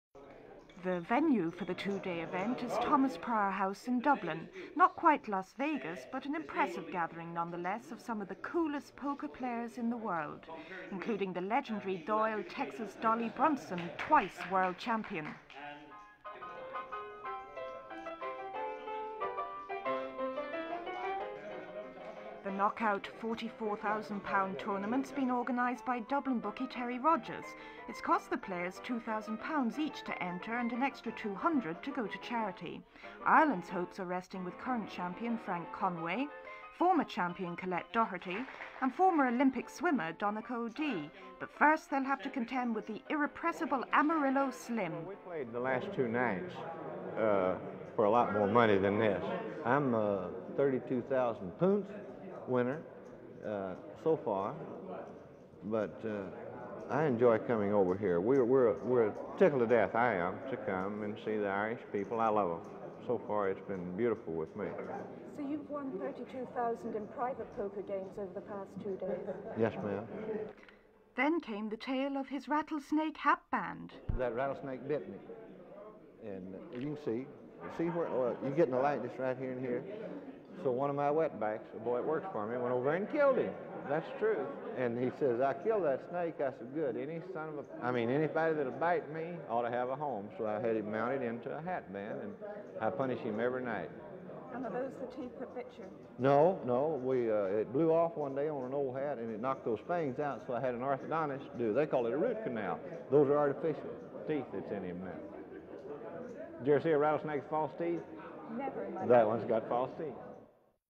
Amarillo Slim Playing Poker in Ireland - 1982
Thomas Prior House in Dublin’s Ballsbridge is the venue for the £44,000 Irish Eccentric International Hold-Em Tournament. White it is not quite Las Vegas, the tournament has attracted some of the coolest poker players in the world including Doyle Texas Dolly Brunson, the 1976 and 1977 champion.

The knockout tournament is organised by Dublin bookmaker and Eccentric Club chairman Terry Rodgers. Each player pays £2,000 to enter and an additional £200 goes to charity.

Ireland’s poker hopes are resting on current champion Frank Conway, 1980 Irish poker champion Collette Doherty and former Olympic swimmer Donnacha O’Dea. However they must contend with the irrepressible Amarillo Slim Preston, who has won £32,000 so far.

Amarillo Slim Preston’s hatband is made from a rattlesnake that dared to bite his hand. When the snake’s original teeth fell out, an orthodontist fitted the snake with artificial fangs,

Did you ever see a rattlesnake with false teeth? That one’s got false teeth.

Report broadcast on 12 September 1982.